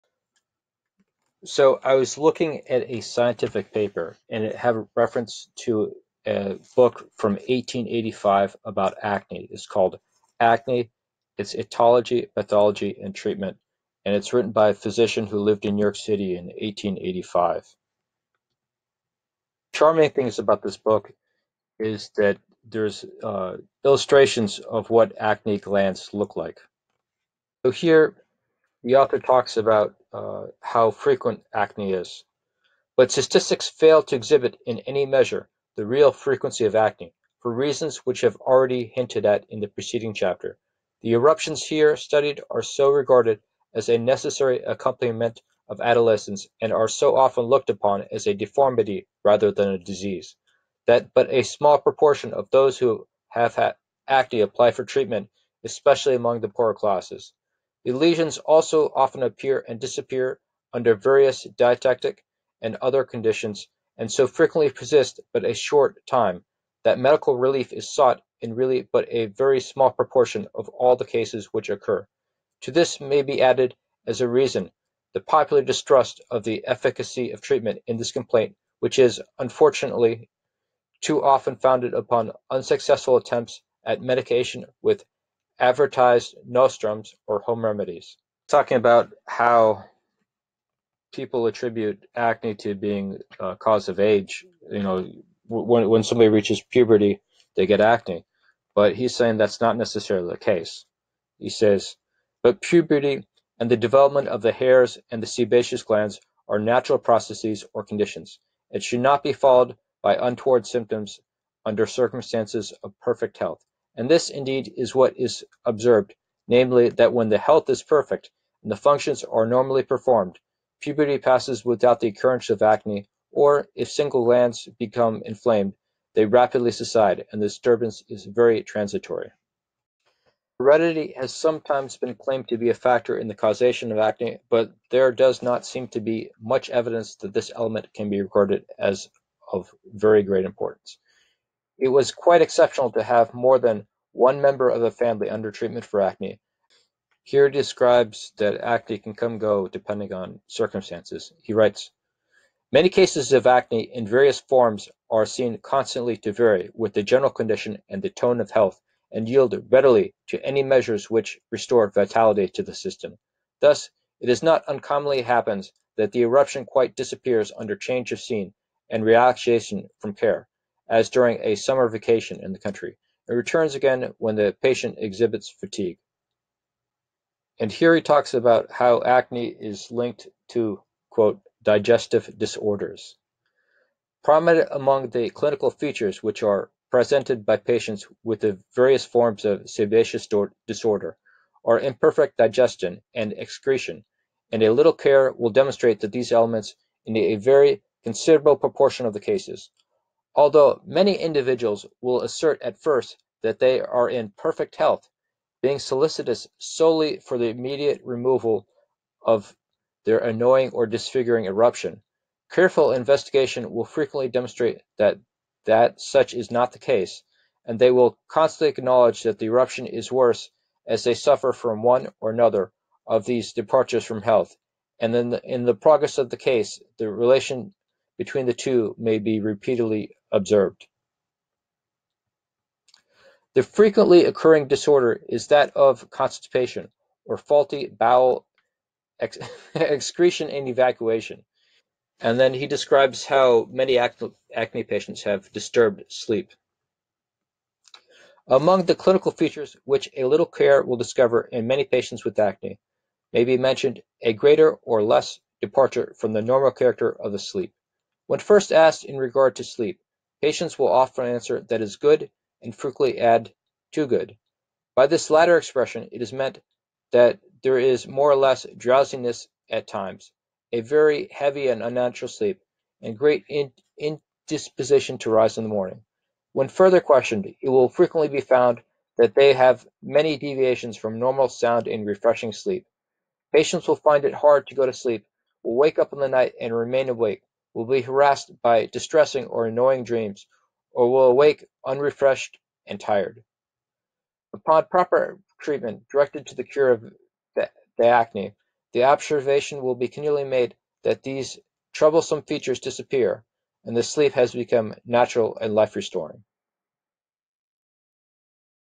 Acne Advice from 1885, part one: Stress, Digestion, Constipation and Sleep
Acne Advice from 1885, part one

Acne has been around for hundreds, if not thousands of years.  A New York doctor, Lucius Duncan Bulkley, wrote a book "Acne: Its Etiology, Pathology and Treatment" in 1885.

This video contains excerpts from Dr. Bulkley observations about acne.  Bulkley notes that acne is widespread and many people have tried ineffectual treatments for it.  Bulkley says that acne is not linked to heredity (that is genetics) or is a natural step in puberty.  He links acne to stress, poor digestion and poor sleep.